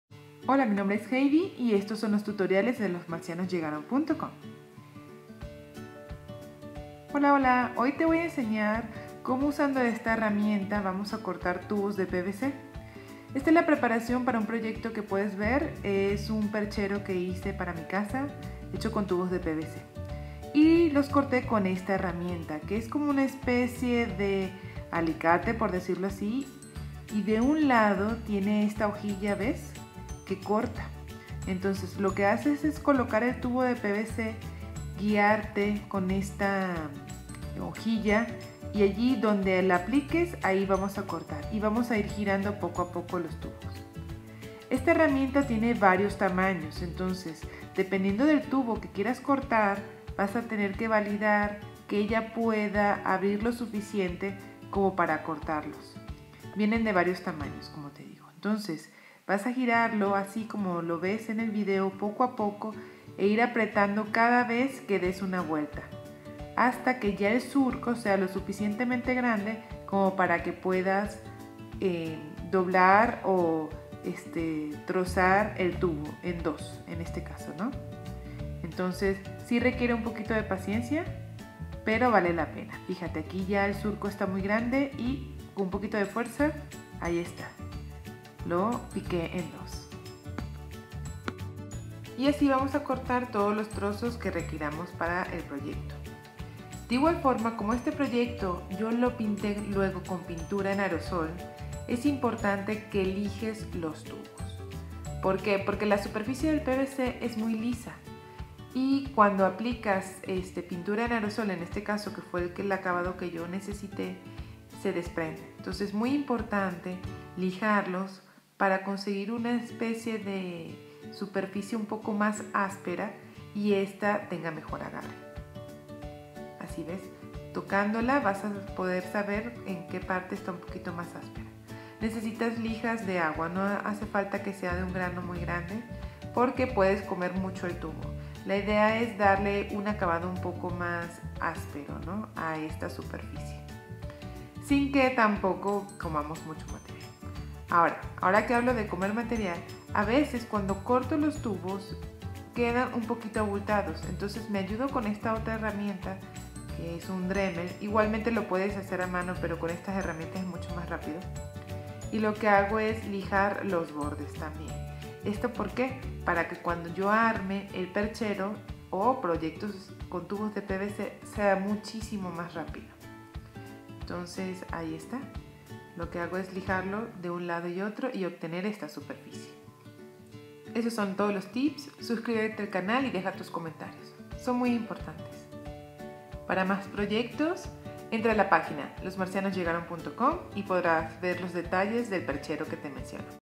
Corte y Lijado de Tuberías PVC
Trabajos previos para la realización de un perchero con tuberías de PVC -- losmarcianosllegaron.com